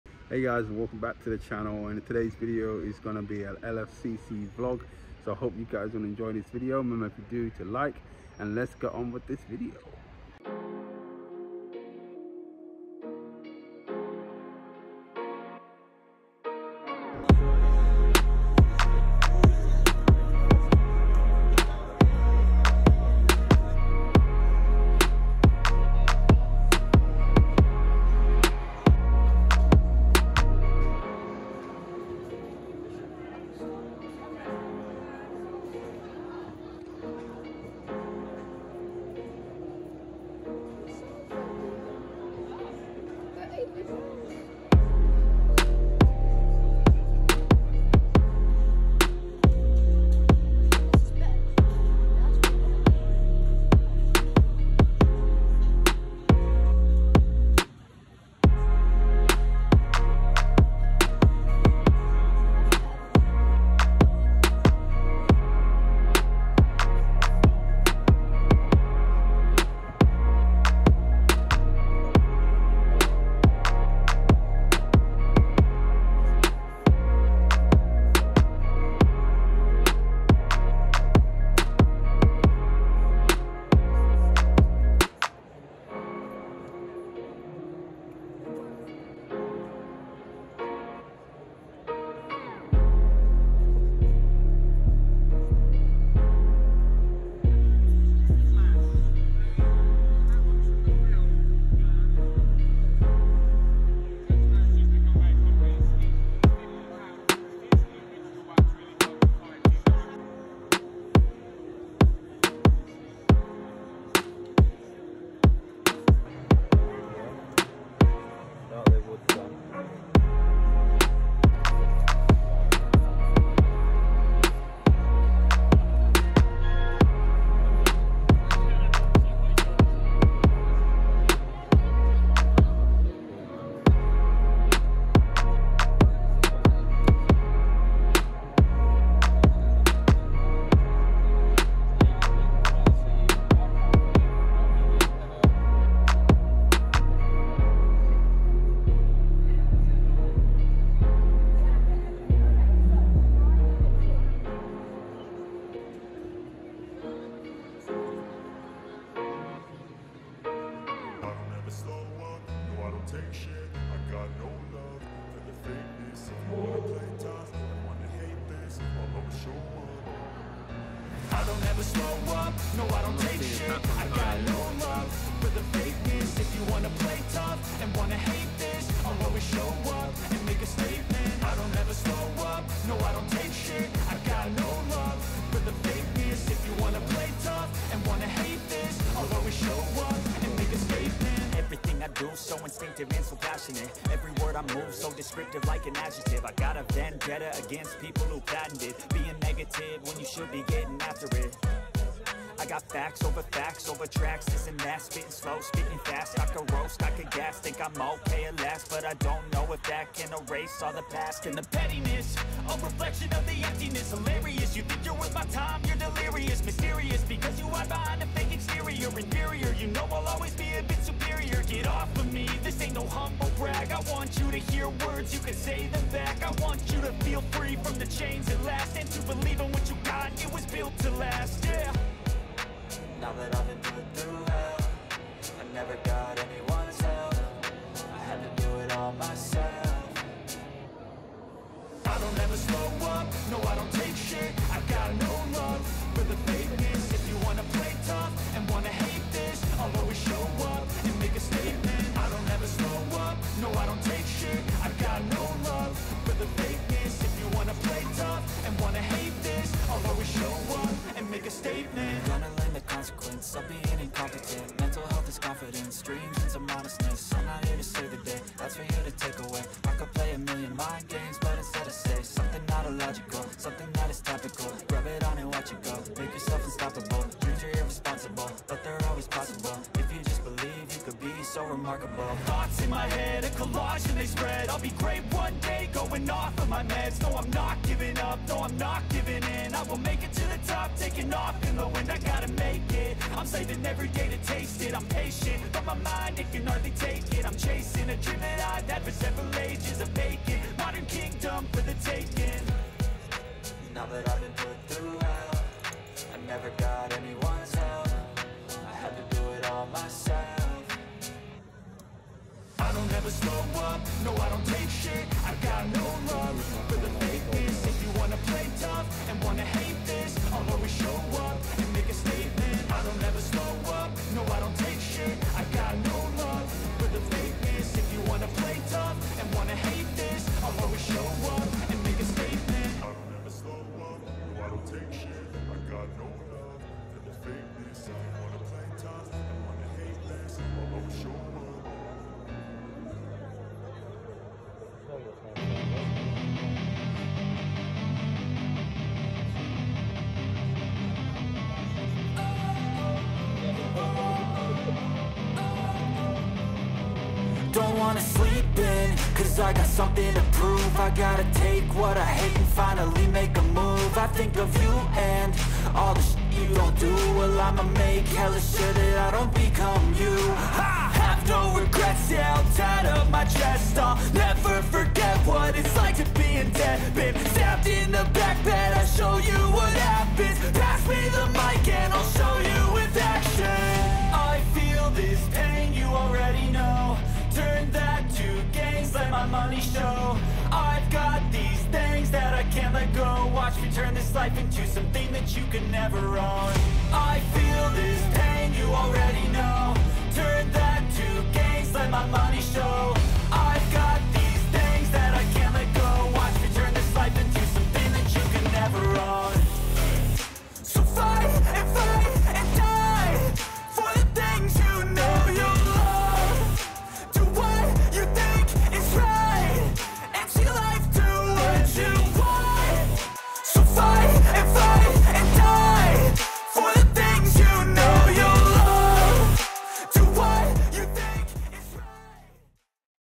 LONDON Film & Comic Con 2022 VLOG!! - PART ONE
Todays video is my vlog from the 2022 summer London Film & Comic Con!

"Live Long & Prosper"